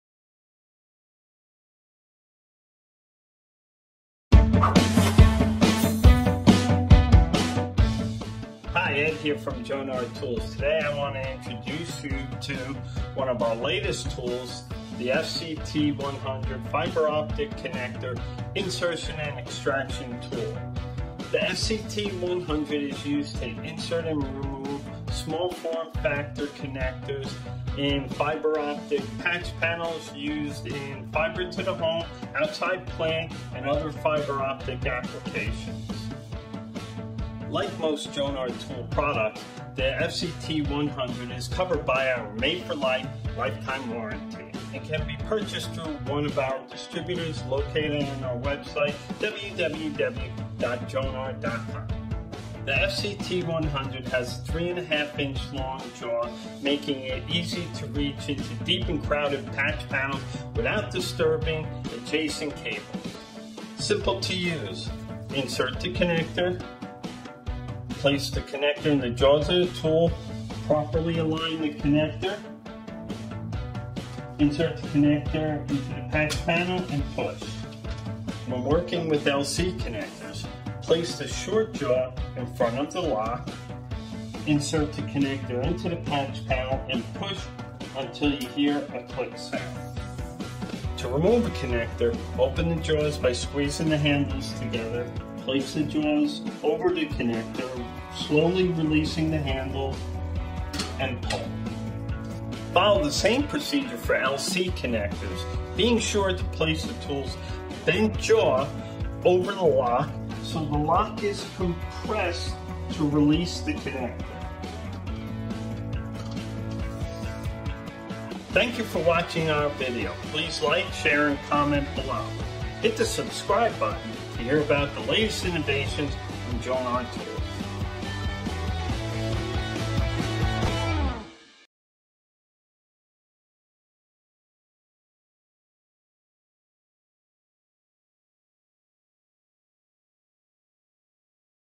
FCT-100 光ファイバコネクタ挿抜用工具
Jonard ToolsのFCT-100は小さな入り組んだパネルへの光ファイバコネクタの挿抜を容易にかつ確実に行える工具です。使い方は簡単です。ぜひ動画をご覧ください。